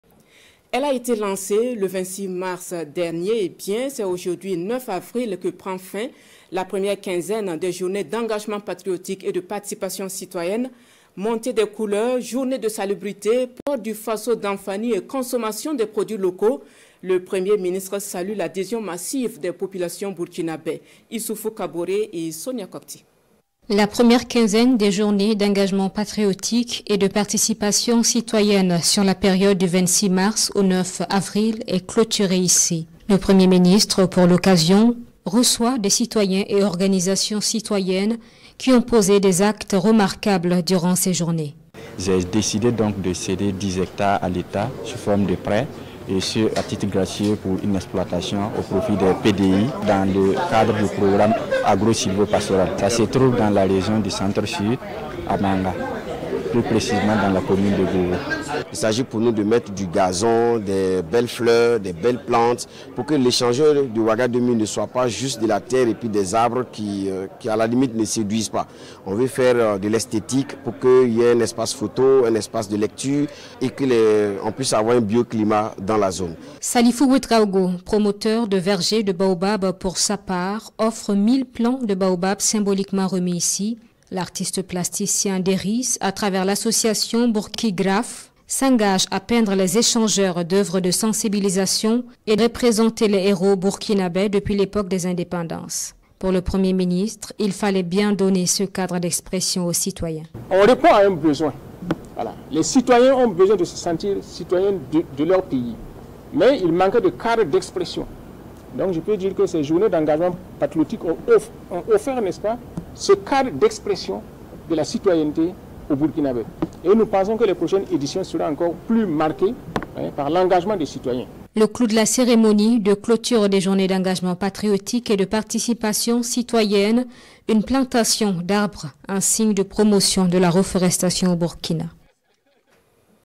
Fin des 1res journées d’engagement patriotique: le 1er ministre salue l'adhésion des populations
Fin des 1res journées d’engagement patriotique : le premier ministre salue l’adhésion massive des populations burkinabè